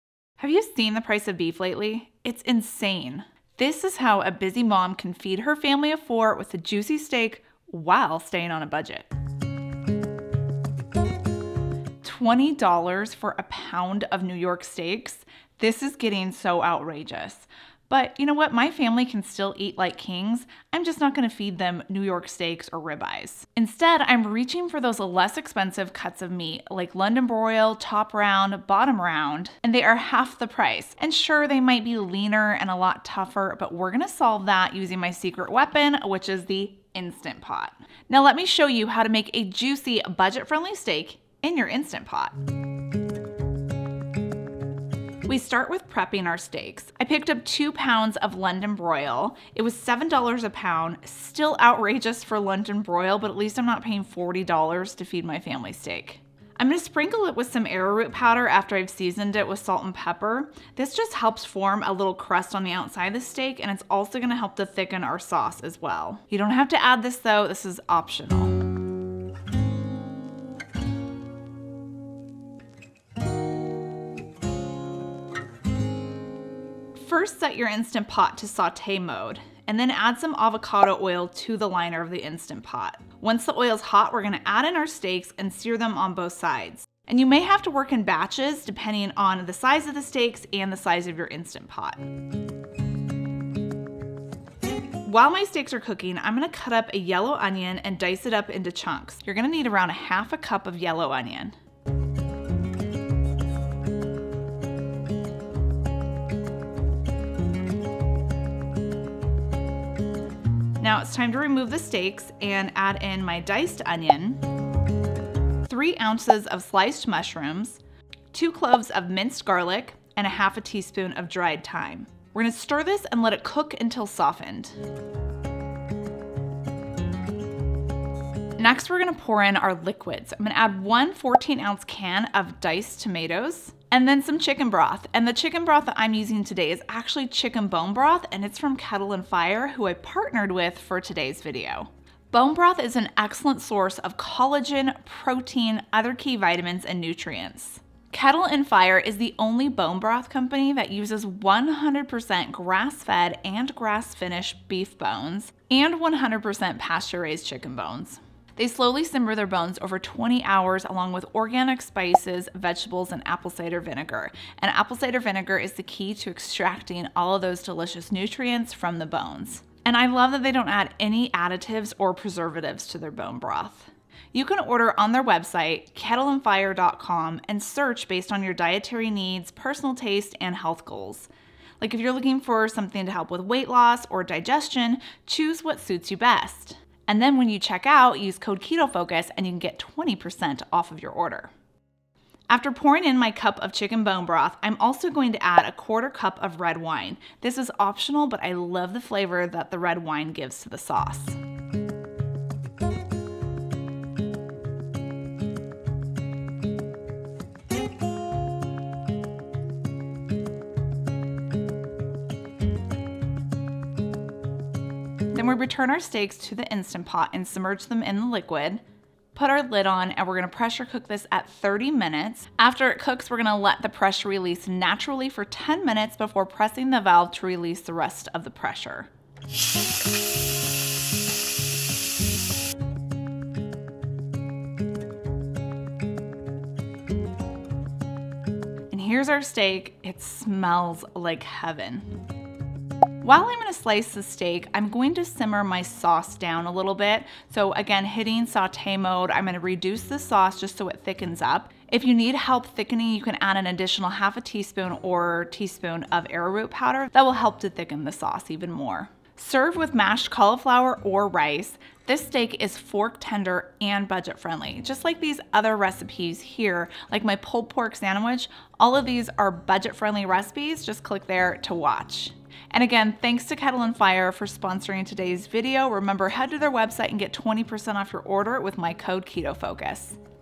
Better than ribeye! My JUICY Keto BUDGET FRIENDLY STEAK
TURN BUDGET FRIENDLY CUTS LIKE LONDON BROIL INTO JUICY STEAK IN YOUR INSTANT POT!

With the rising cost of beef, I'm turning to less expensive cuts of steak like london broil, top round and bottom round steak. These cuts tend to be tougher, but when you cook them in the Instant Pot with a little bit of tomato gravy, they become fork tender.

⬇️ PRODUCTS MENTIONED ⬇️
Arrowroot Powder
Instant Pot

INSTANT POT STEAK
Serves: 6

2-3 pounds london broil, top round, bottom round steak or roast
1 teaspoon arrowroot powder, optional
2 tablespoons avocado oil
1/2 cup diced onion
3 oz sliced mushrooms
2 cloves garlic, minced
1/2 teaspoon dried thyme
14 oz can diced tomatoes, drained
1 cup chicken broth
1/4 cup red wine, optional

1️⃣ 00:42 - Pat steaks or roast dry with a paper towel. Season generously with salt and pepper. Dust with arrowroot powder or your favorite thickener if using.
2️⃣ 01:16 - Set Instant Pot to sauté mode. Add avocado oil. Once oil is heated through, add steaks to the liner of the Instant Pot. Do not crowd the steaks, leave room for steam to escape in order to get a proper sear and crust on the outside of each steak. You may have to work in batches. Sear on all sides. Remove steaks and set aside.
3️⃣ 01:53 - To the liner of the Instant Pot, add diced onions, mushrooms, garlic and thyme. Sauté until almost softened. Stir in diced tomatoes, chicken broth and red wine, if using.
4️⃣ 03:27 - Return steaks to the Instant Pot and submerge within the liquid. Place the lid on the pressure cooker and turn the vent to seal. Pressure cool on high for 30 minutes. Let pressure release naturally for 10 minutes before sliding or pressing the nob to vent remaining pressure.
5️⃣ 03:53 - Remove steak. To thicken the sauce, turn the Instant Pot to sauté mode and let sauce reduce. Add more arrowroot powder or your favorite thickener if needed. Pour sauce over steak once finished.

Calories 364
Fat 7.4g
Protein 51g
Total Carbs 5.9g